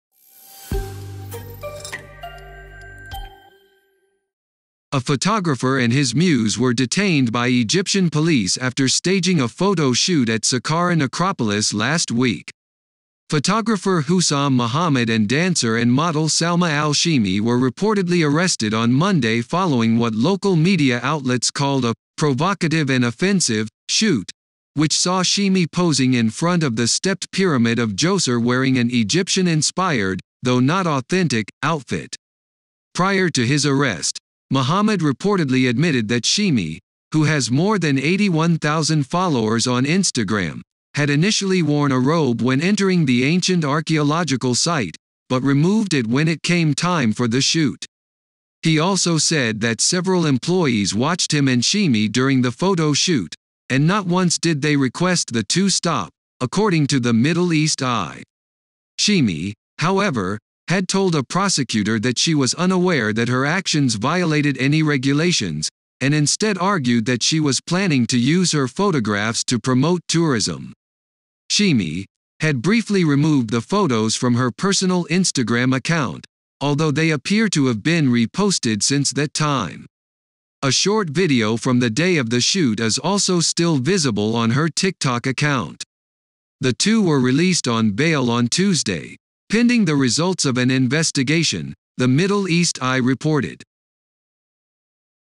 Photographer and model arrested by Egyptian police following photo shoot at ancient site
A photographer and his muse were detained by Egyptian police after staging a photo shoot at Saqqara necropolis last week.